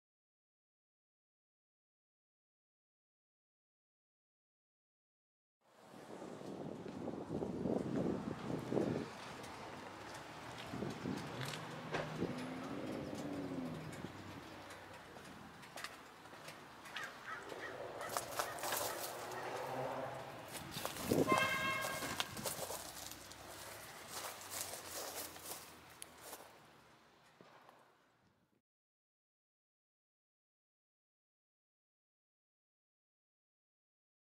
BMW M3 (E36) - Alice's Restaurant - San Francisco
A BMW M3 (E36) parked at Alice's Restaurant near San Francisco (USA).

- 286 HP
- 320 Nm max. torque
- 250 km/h topspeed
- 6.0 sec. (0 to 100 km/h)